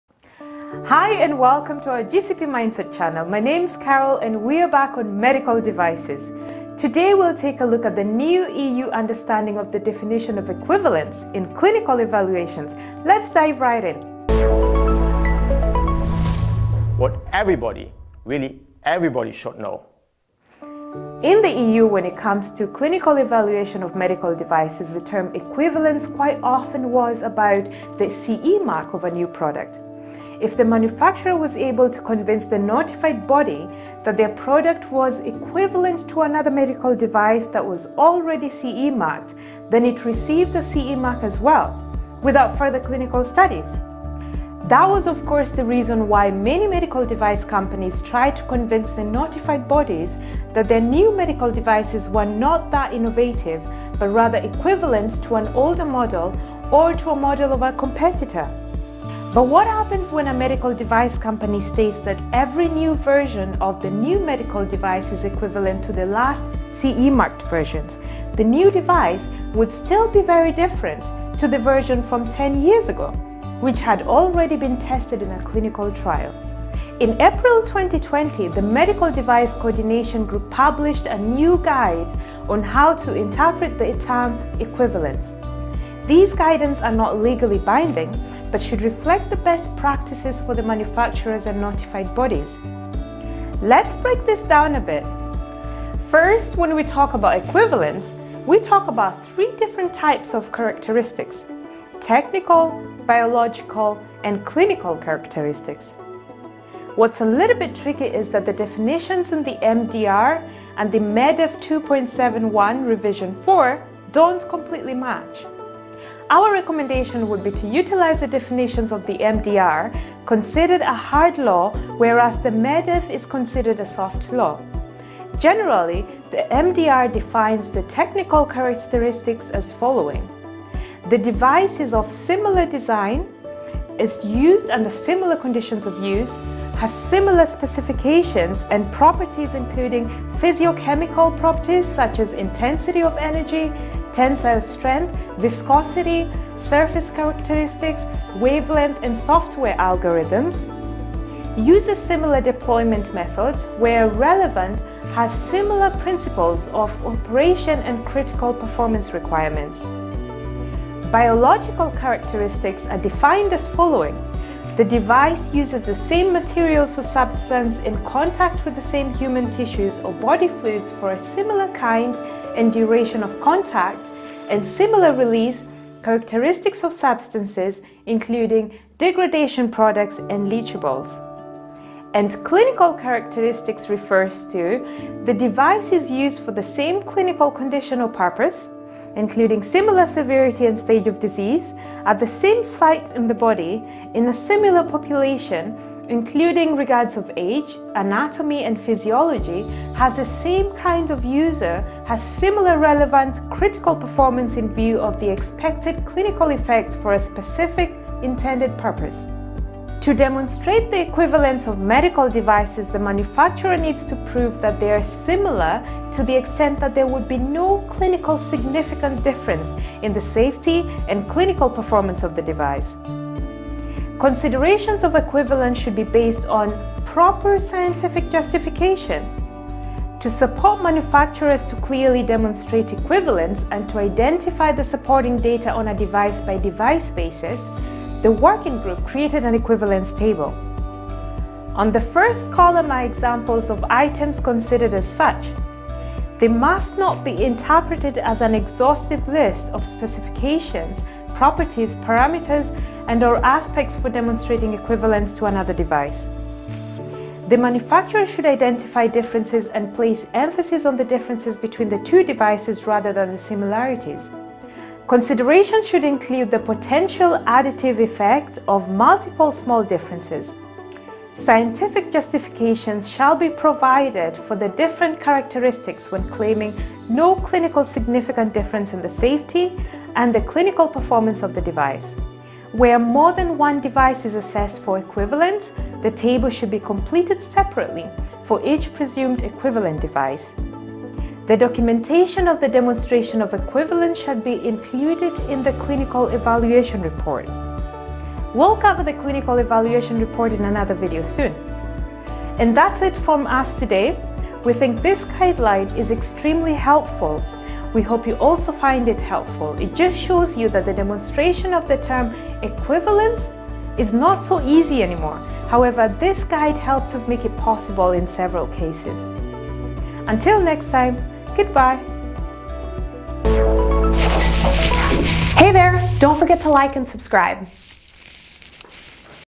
Basics - Part 24 - Equivalence in Clinical Evaluations
Without clinical trials, we wouldn’t have any vaccines, treatments for cancer, heart disease, diabetes and many others. What should everybody know about Clinical Trials? How are pharmaceuticals and medical devices developed? This is our new video of our series about the basics of clinical trials to give you some answers. Part 24 - Equivalence in Clinical Evaluations is presented by our expert Carol Lazovic.